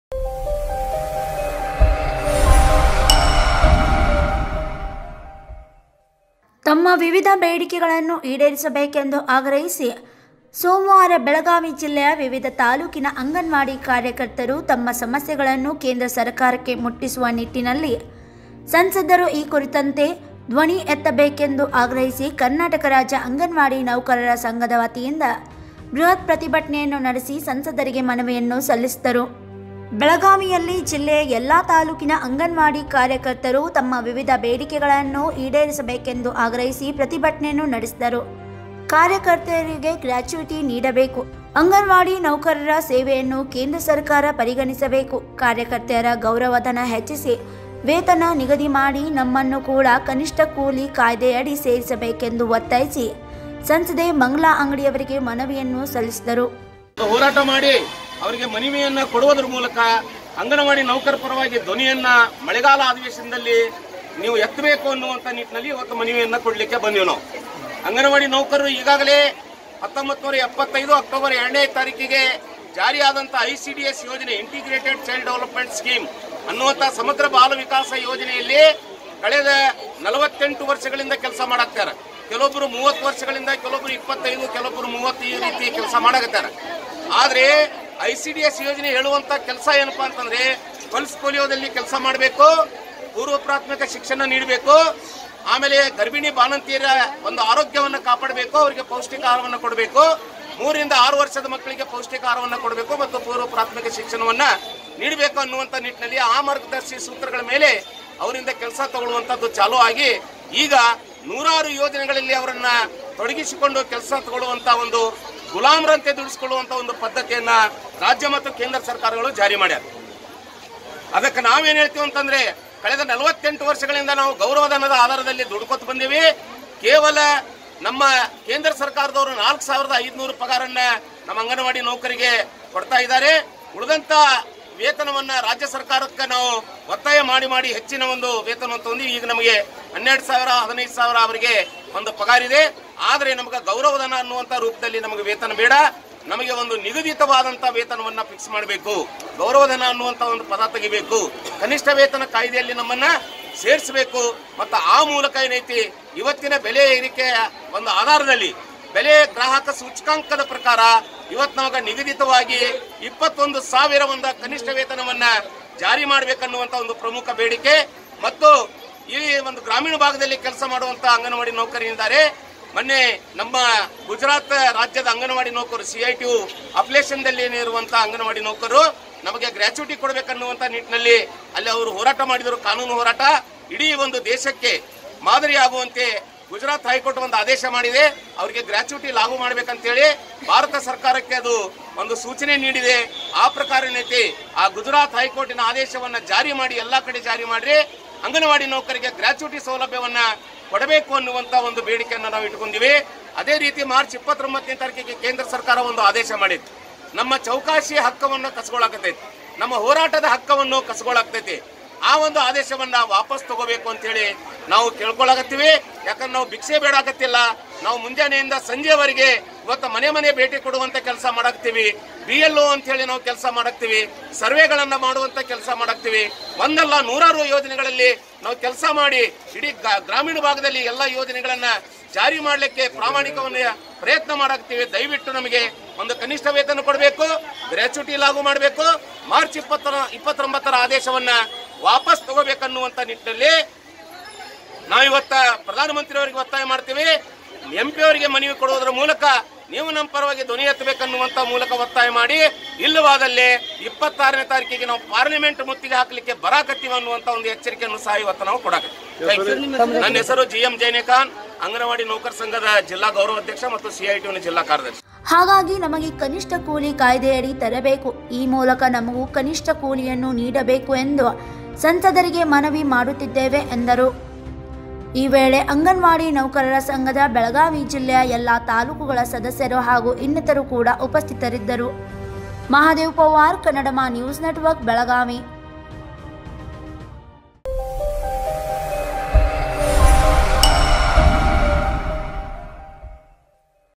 Massive Protest by Karnataka State Anganwadi Employees Union Demand to Meet Various Demands
ಕರ್ನಾಟಕ ರಾಜ್ಯ ಅಂಗನವಾಡಿ ನೌಕರರ ಸಂಘದಿAದ ಬೃಹತ್ ಪ್ರತಿಭಟನೆ
ವಿವಿಧ ಬೇಡಿಕೆಗಳನ್ನು ಈಡೇರಿಸುವಂತೆ ಆಗ್ರಹ
ಅಂಗನವಾಡಿ ಕಾರ್ಯಕರ್ತೆಯರಿಂದ ಸಂಸದರಿಗೆ ಮನವಿ
ಕಾರ್ಯಕರ್ತೆಯರ ಸಮಸ್ಯೆಗಳನ್ನು ಕೇಂದ್ರ ಸರ್ಕಾರಕ್ಕೆ ಮುಟ್ಟಿಸುವಂತೆ ಮನವಿ
ಕಾರ್ಯಕರ್ತೆಯರ ಗೌರವ ಧನ ಹೆಚ್ಚಿಸಿ ವೇತನ ನಿಗದಿ ಮಾಡುವಂತೆ ಬೇಡಿಕೆ
ಸಂಸದರ ಮನೆ ಹಾಗೂ ಕಚೇರಿ ಮುಂದೆ ಹೋರಾಟ ನಡೆಸಿದ ಕಾರ್ಯಕರ್ತೆಯರು
ಮಳೆಗಾಲದ ಅಧಿವೇಶನದಲ್ಲಿ ಬೇಡಿಕೆ ಕುರಿತು ಧ್ವನಿ ಎತ್ತುವಂತೆ ಒತ್ತಾಯ